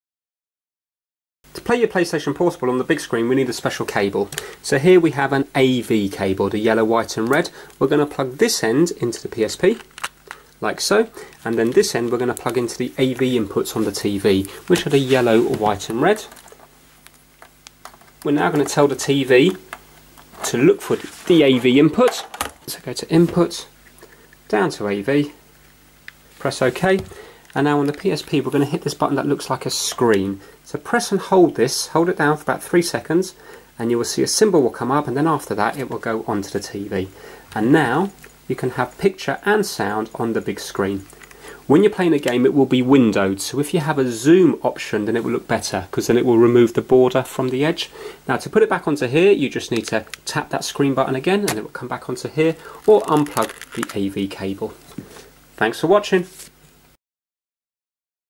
How to Connect a PSP 2000 / 3000 to a TV via AV
Hi, this video shows you how to play your PlayStation Portable (PSP 2000 or 3000) on a television via an AV Composite connection (yellow, white & red).